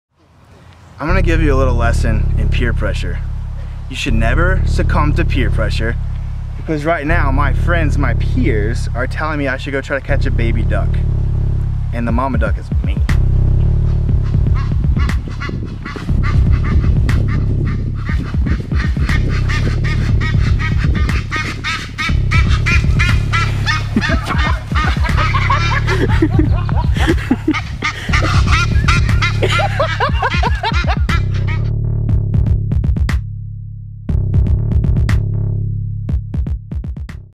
Egyptian Goose Attack
CatchingCreation.com
While hiking around Crandon Park in Key Biscayne we learned how aggressive the Egyptian Geese were in regards to protecting their young